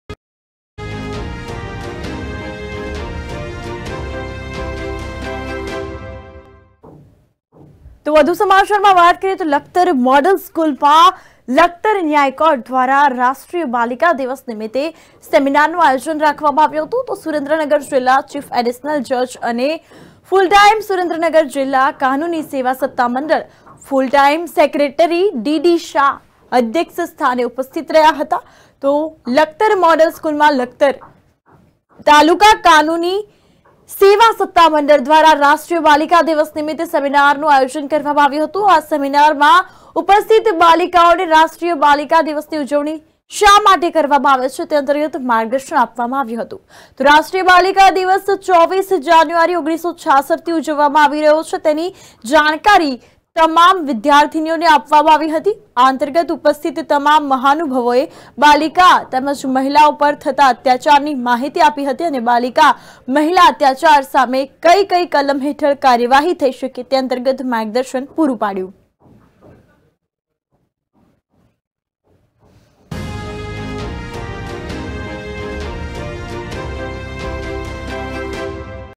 LAKHATAR : લખતર ન્યાયકોર્ટ દ્વારા રાષ્ટ્રીય બાલિકા દિવસ નિમીતે સેમિનારનું આયોજન કરાયુ
Top Headlines
News Flash